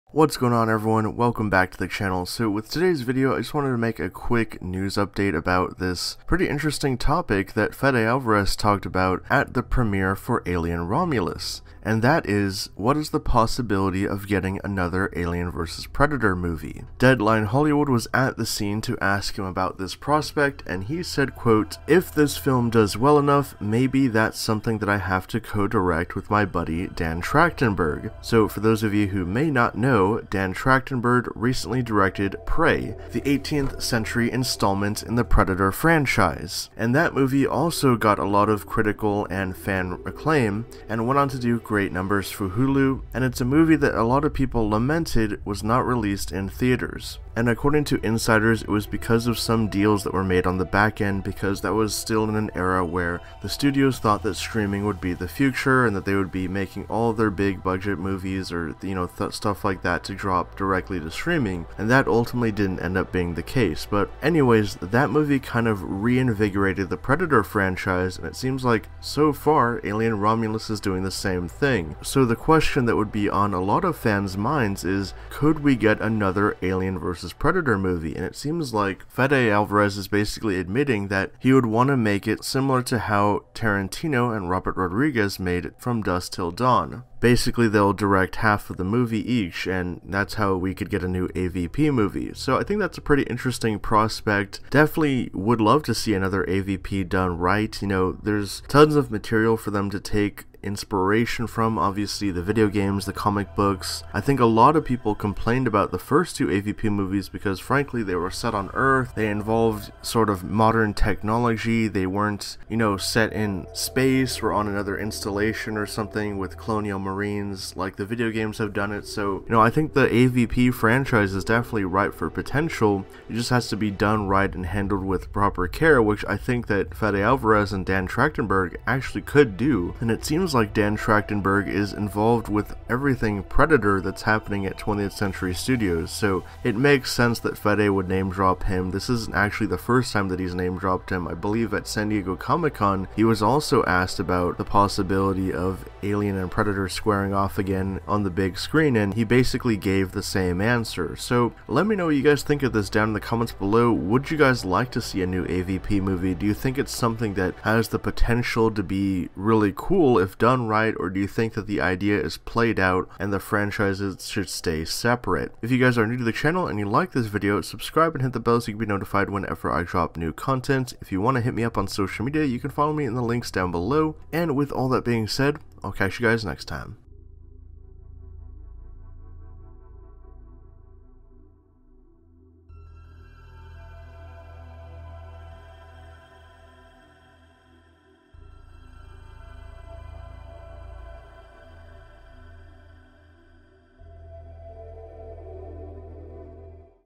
Alien Romulus Director Reveals His Pitch For New Alien vs Predator Movie!
Fede Alvarez was about tackling Alien vs Predator at the premiere for his film Alien Romulus, and he gave a specific pitch on how to do it!